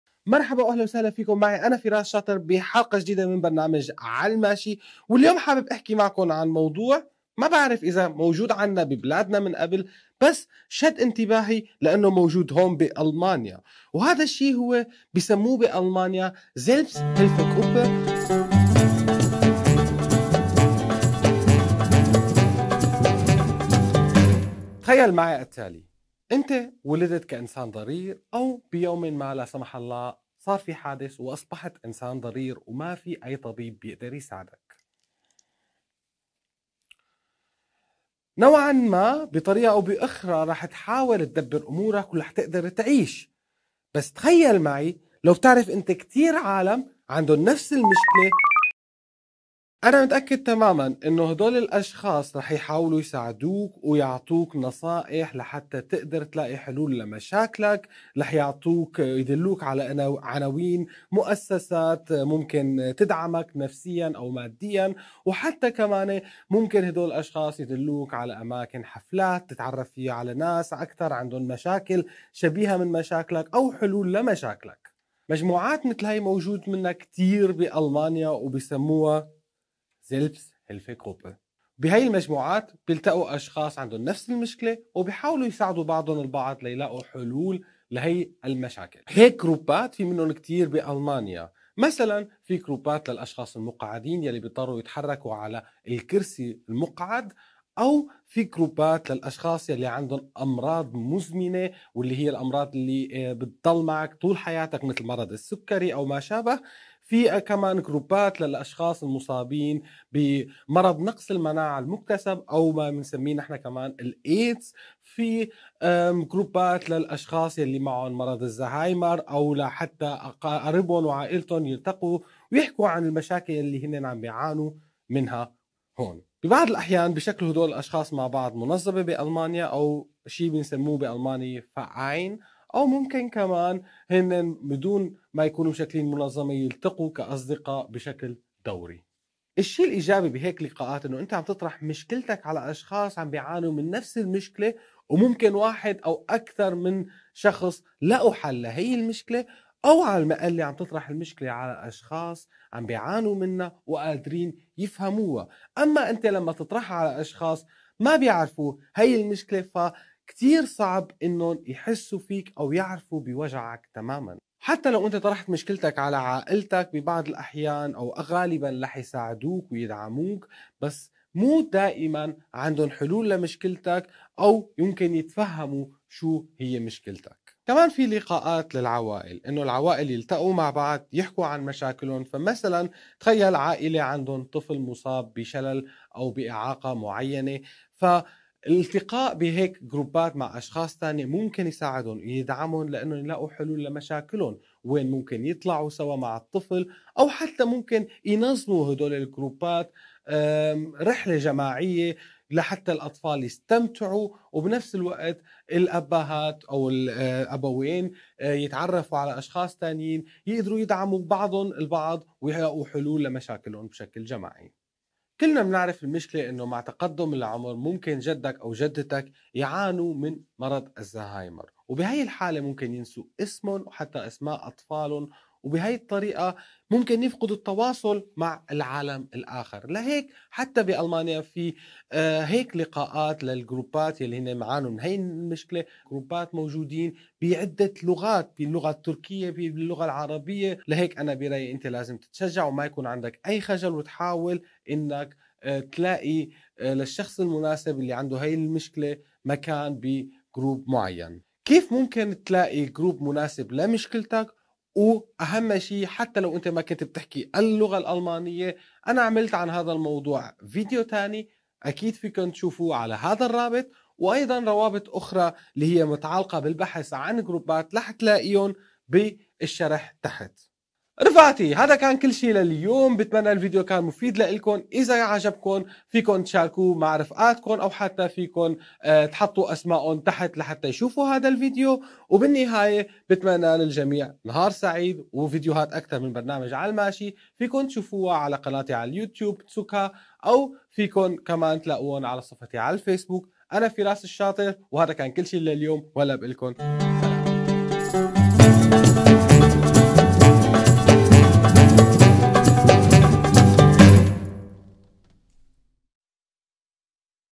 3AL Maashi - Selbsthilfe
In der heutigen Folge von 3AL Maashi erklärt Firas Euch was Selbsthilfegruppen sind.

Selbsthilfegruppen - so ein Wort gibt es nicht in der arabischen Sprache. Hier in Deutschland sind sie aber sehr verbreitet und helfen Menschen mit ähnlichen Problematiken zusammen zu kommen.

Was genau das bedeutet erfährst Du in der Folge!

في حلقة اليوم من برنامج "ع الماشي" سأتحدث عن مجموعات الدعم الذاتي.

مجموعات الدعم الذاتي- لا تستخدم هذه الكلمة بكثرة في اللغة العربية. اما في المانيا فهي واسعة الانتشار فعن طريق هذه المجموعات يساعد الاشخاص بعضهم البعض في ايجاد حلول لما يعانون منه.

ماذا يعني ذلك؟ ستعرف في هذه الحلقة!

No commercial & use without licencing.
Short citations in TV up to 10 sec are free.
NonProfit use & Education/schools is free.